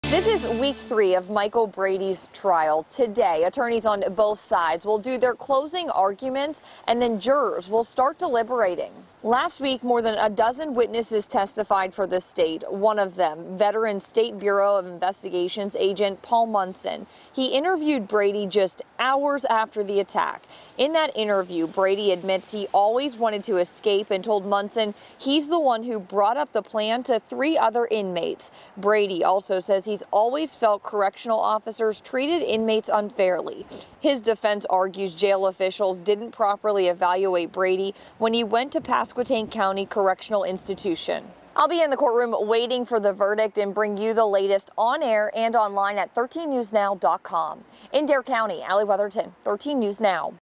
Week 3 deadly Pasquotank County jail break trial: Lawyers to present closing arguments
Lawyers in the Mikel Brady trial will present their closing arguments. After the closing arguments, the jury will begin deliberating.